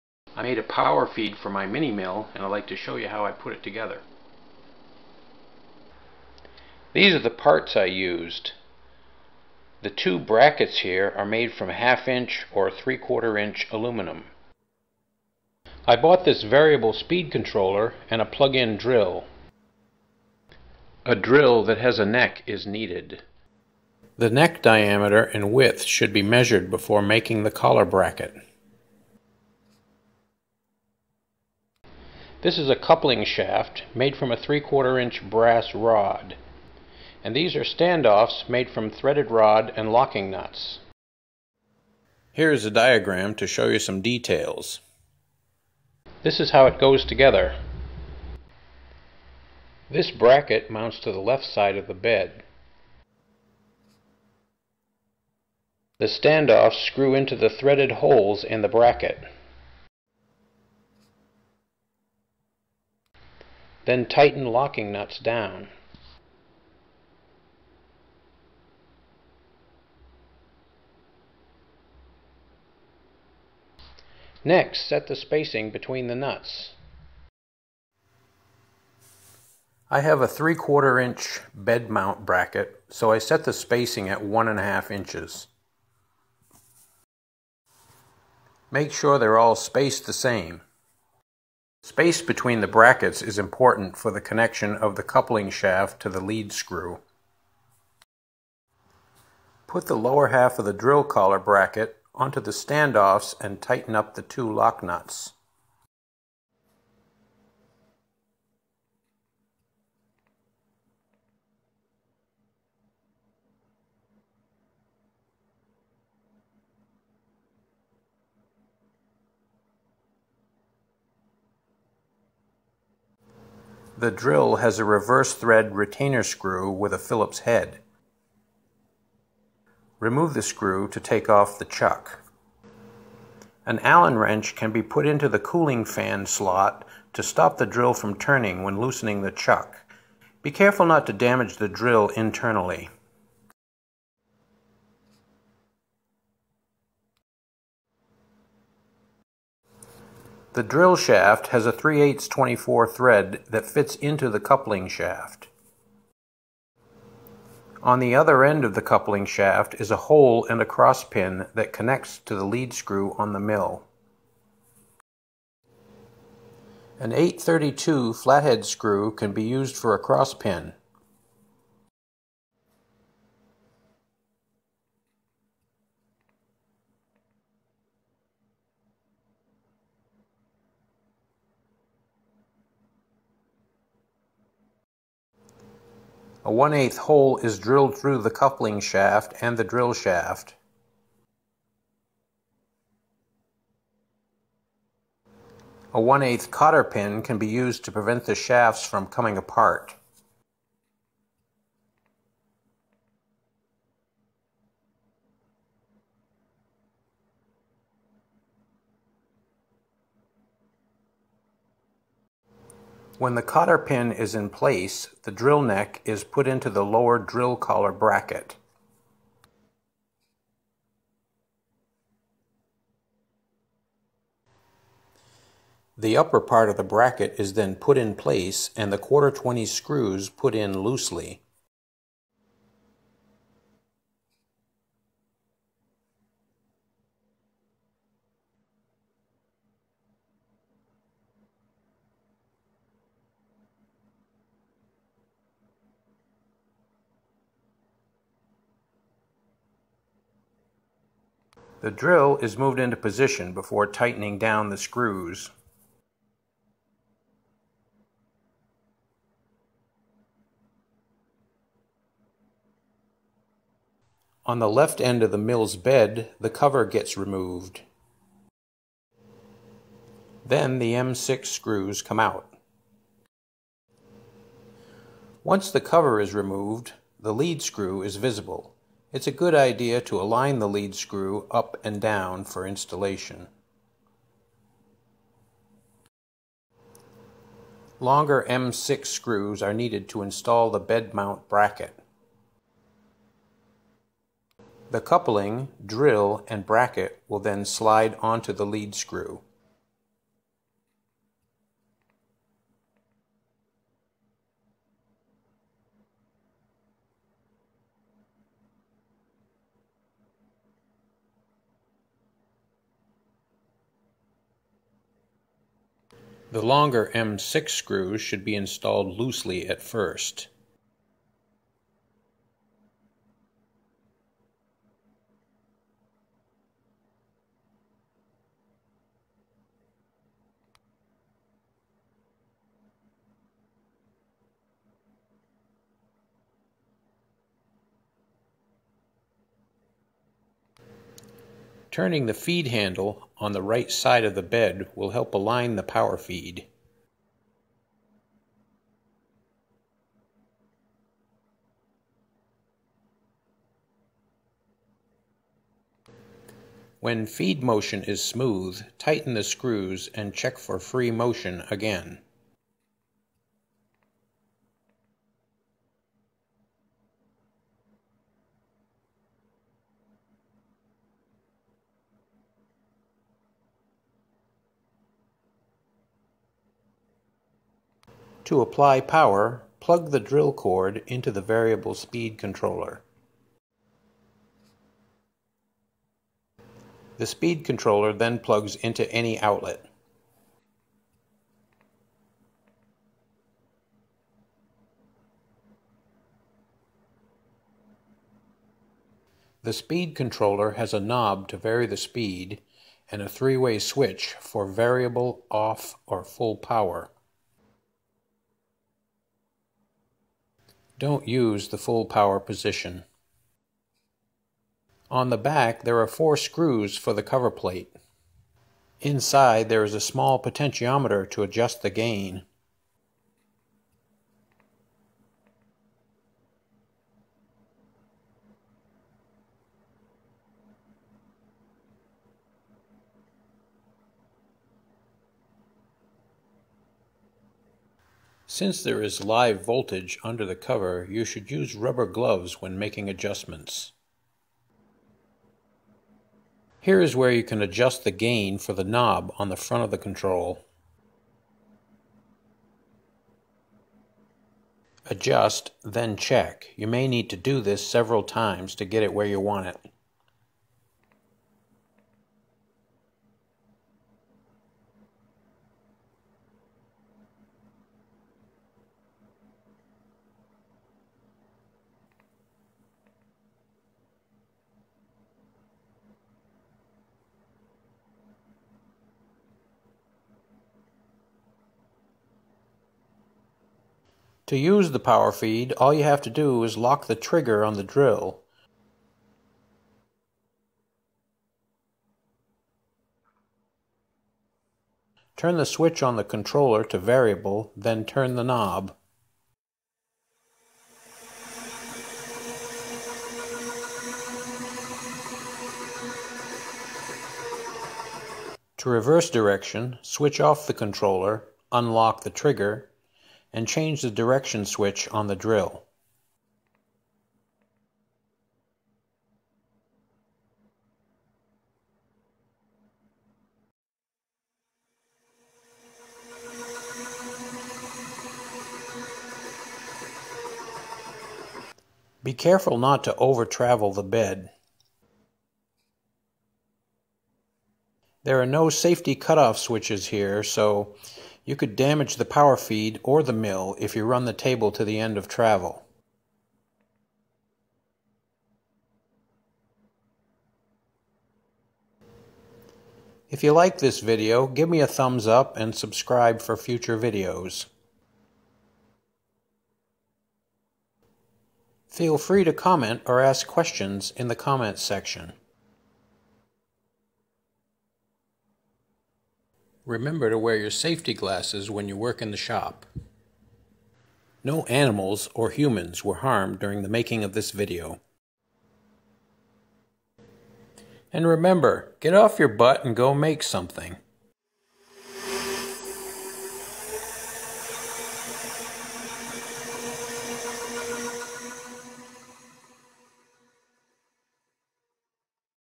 POWER FEED FOR A MINI-MILL
Milling machine productivity can be greatly increased by using a power feed. Even a small mill can be used to make it's own auxiliary parts. This video is about the Power Feed I built for my Sieg built mini-mill. The milling machine, drill, and speed controller are available from Harbor Freight Tools

Central Machinery 2 speed benchtop mill/drill (44991)

3/8" corded drill
Drill Master 3/8 variable speed reversible drill (60615)

Drill Master router speed controller (43060)